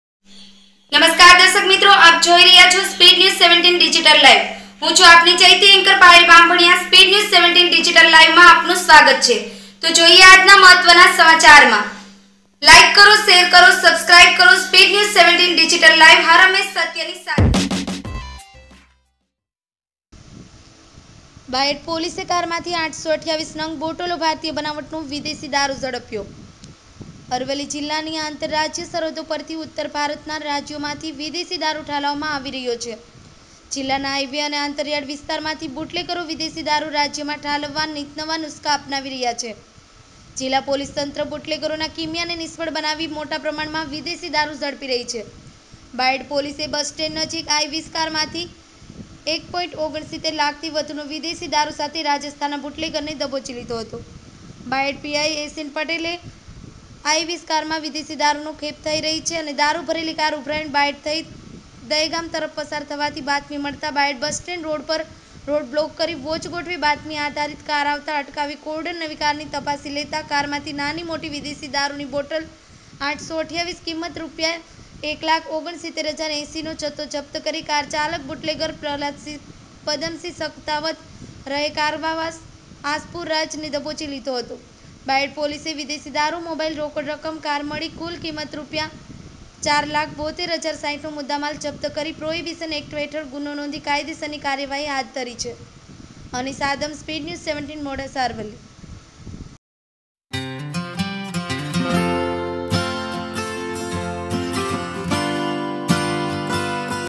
ARAVALLI બાયડ પોલીસે કારમાંથી 828 નંગ બોટલો ભારતીય બનાવટનો વિદેશી દારૂ ઝડપ્યો.
બાયડ પોલીસે કારમાંથી 828 નંગ બોટલો ભારતીય બનાવટનો વિદેશી દારૂ ઝડપ્યો.

અરવલ્લી જીલ્લાની આંતરરાજ્ય સરહદો પરથી ઉત્તર ભારતના રાજ્યોમાંથી વિદેશી દારૂ ઠાલવવામાં આવી રહ્યો છે. જીલ્લાના હાઇવે અને અંતરિયાળ વિસ્તારમાંથી બુટલેગરો વિદેશી દારૂ રાજ્યમાં ઠાલવવા નિતનવા નુસ્ખા અપનાવી રહ્યા છે. જીલ્લા પોલીસતંત્ર બુટલેગરોના કીમિયાને નિષ્ફળ બનાવી બનાવી મોટા પ્રમાણમાં વિદેશી દારૂ ઝડપી રહી છે. બાયડ પોલીસે બસ સ્ટેન્ડ નજીકથી આઈ-૨૦ કારમાંથી ૧.૬૯ લાખથી વધુનો વિદેશી દારૂ સાથે રાજ્સ્થાનના બુટલેગરને દબોચી લીધો હતો.

બાયડ પીઆઈ એસ.એન.પટેલને આઈ-૨૦ કારમાં વિદેશી દારૂની ખેપ થઇ રહી છે. દારૂ ભરેલી કાર ઉભરાણ બાયડ થઇ દહેગામ તરફ પસાર થવાની બાતમી મળતા બાયડ બસ સ્ટેન્ડ રોડ પર રોડ બ્લોક કરી વોચ ગોઠવી બાતમી આધારિત આઈ-૨૦ કાર આવતા અટકાવી કોર્ડન કરી કારની તલાસી લેતા કારમાંથી નાની-મોટી વિદેશી દારૂની બોટલ-૮૨૮ કીં.રૂ.૧૬૯૦૮૦/- નો જથ્થો જપ્ત કરી કાર ચાલક બુટલેગર પ્રહલાદસિંહ પદમસિંહ સકતાવત (રહે,કારવાખાસ-આસપુર,રાજ) ને દબોચી લીધો હતો. બાયડ પોલીસે વિદેશી દારૂ,મોબાઈલ,રોકડ રકમ,કાર મળી કુલ.રૂ.૪૭૨૦૬૦/- નો મુદ્દામાલ જપ્ત કરી પ્રોહીબીશન એક્ટ હેઠળ ગુન્હો નોંધી કાયદેસરની કાર્યવાહી હાથધરી હતી.
રિપોર્ટર અનીસ આદમ મોડાસા અરવલ્લી